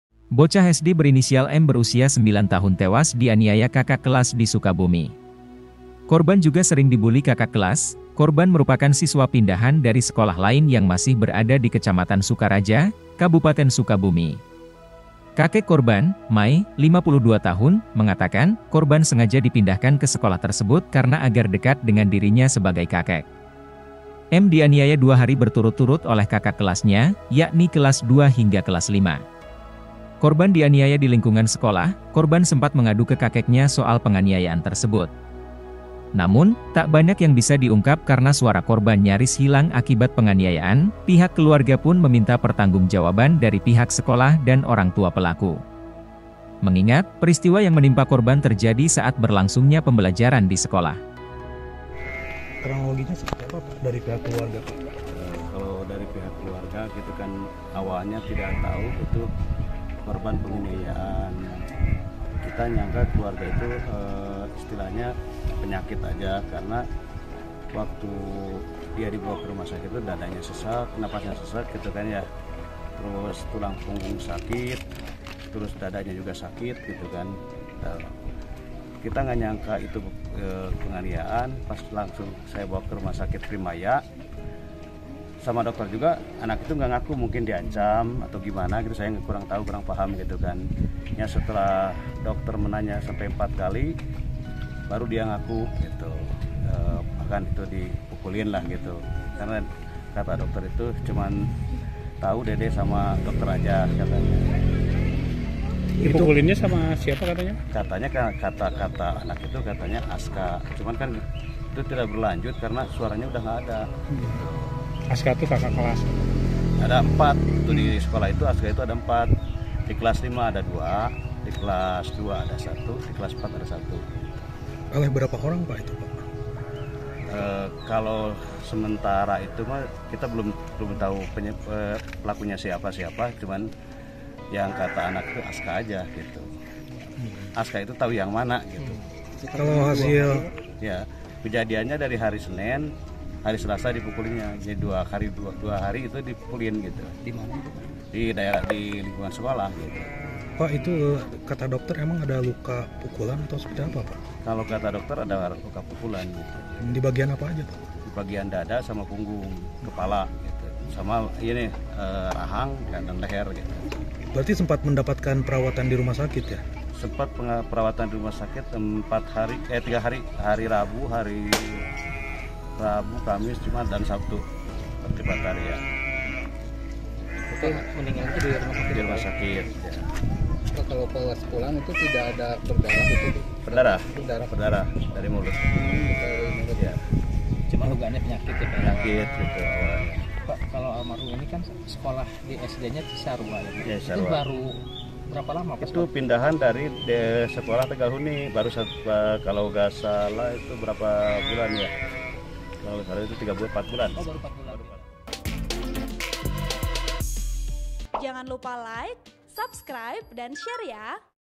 KRONOLOGI BOCAH SD DI SUKABUMI TEWAS DIANIAYA KAKAK KELAS, SUARA KORBAN SEMPAT HILANG
TRIBUNJABARVIDEO- BOCAH SD berinisial M berusia 9 tahun tewas dianiaya kakak kelas di Sukabumi. Korban juga sering dibully kakak kelas.

Korban merupakan siswa pindahan dari sekolah lain yang masih berada di Kecamatan Sukaraja, Kabupaten Sukabumi.

Kakek korban, MY, 52 tahun, mengatakan, korban sengaja dipindahkan ke sekolah tersebut karena agar dekat dengan dirinya sebagai kakek.

M dianiaya dua hari berturut-turut oleh kakak kelasnya, yakni kelas 2 hingga kelas 5.   Korban dianiaya di lingkungan sekolah.

Korban sempat mengadu ke kakeknya soal penganiayaan tersebut. Namun, tak banyak yang bisa diungkap karena suara korban nyaris hilang akibat penganiayaan.

Pihak keluarga pun meminta pertanggungjawaban dari pihak sekolah dan orang tua pelaku.

Mengingat, peristiwa yang menimpa korban terjadi saat berlangsungnya pembelajaran di sekolah.